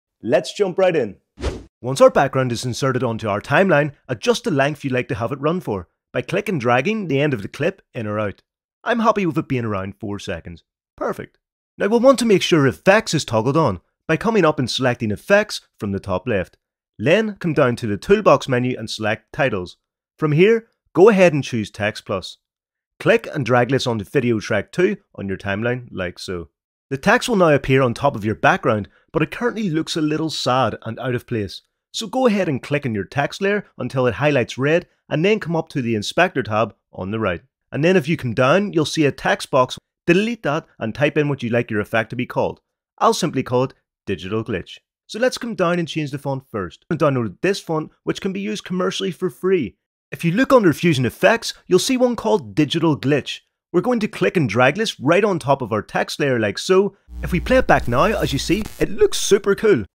FAST AND EASY GLITCH EFFECT You NEED to TRY Next…
FAST AND EASY GLITCH EFFECT You NEED to TRY Next… (FREE EFFECT)

In this video, I'll show you how to create glitch text effect in DaVinci Resolve. This is one of the DaVinci Resolve quick text effects that's actually very easy to do but looks great!.

If you're a beginner or just starting out, this DaVinci Resolve tutorial is for you! This effect is part of the FREE version of DaVinci Resolve, so you don't have to spend any money to learn how to create it. After watching this video, you'll be able to create your own text glitch effect in no time!